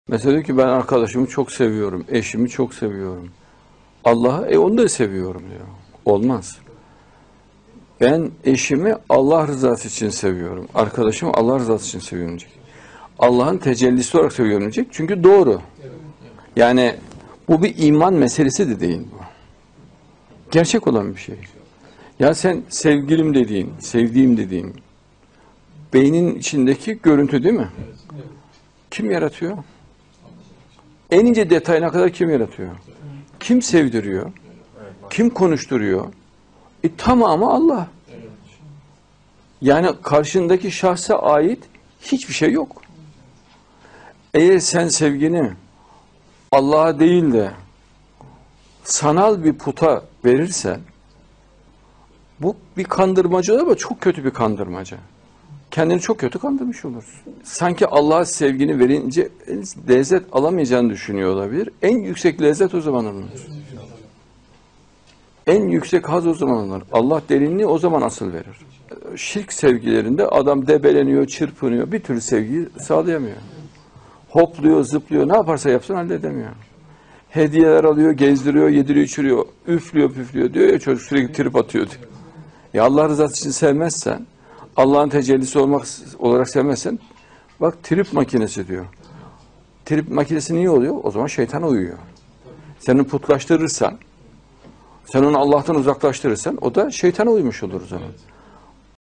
En yüksek haz ve sevgi, Allah sevildiğinde meydana gelir.
Her şey Allah'ın tecellisi olarak sevilir. En yüksek haz ve sevgi, Allah sevildiğinde meydana gelir. Sn. Adnan Oktar